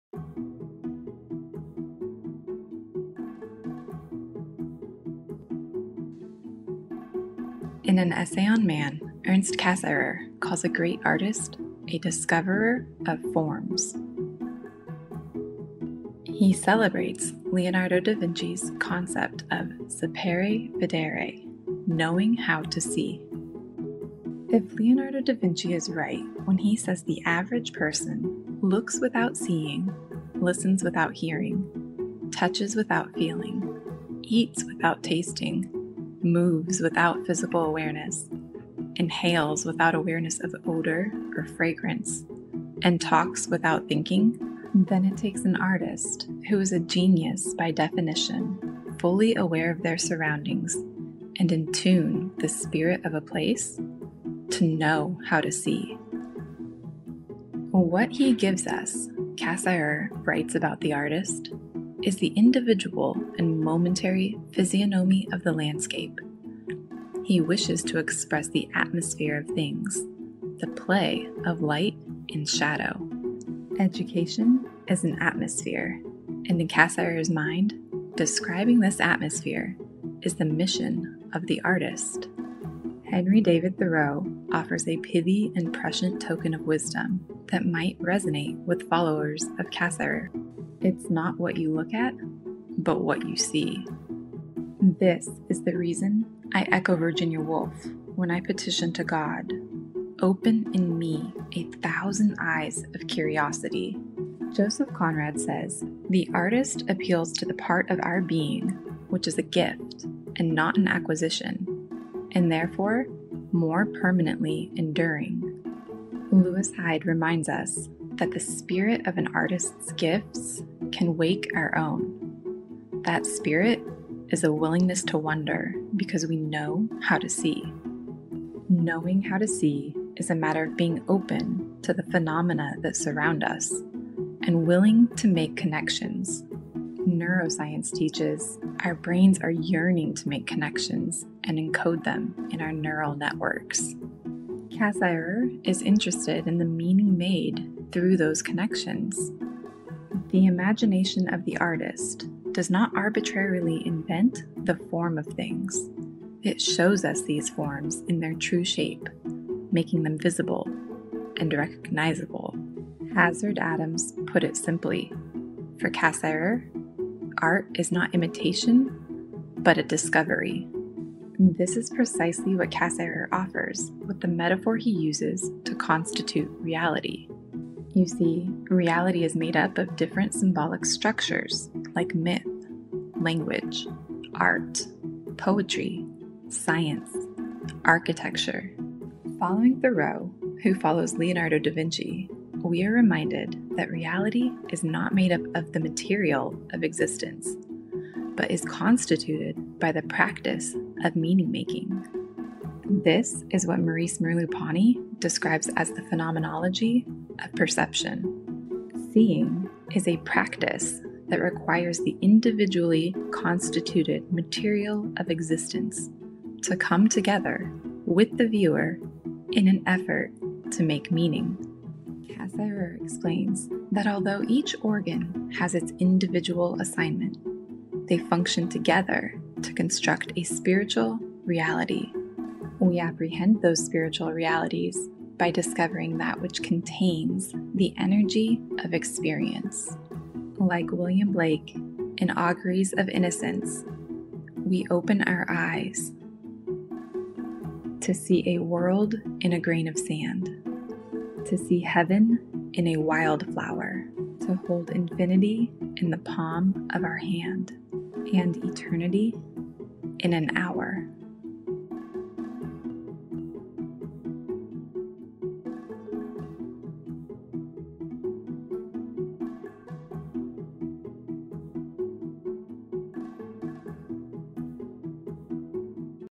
Ernst Cassirer on the Gift of the Artist
This creative composition focuses on the German philosopher, Ernst Cassirer, and his insights that speak to the gift of the artist. This idea is explored by Lewis Hyde in his famous exploration of the artist's community and the gift. The artist, writer, reader, and architect is a visionary - a seer - and their imagination offers the viewer a precious and enduring gift: insight into the container that holds the energy. The artist shows us the connections between phenomena.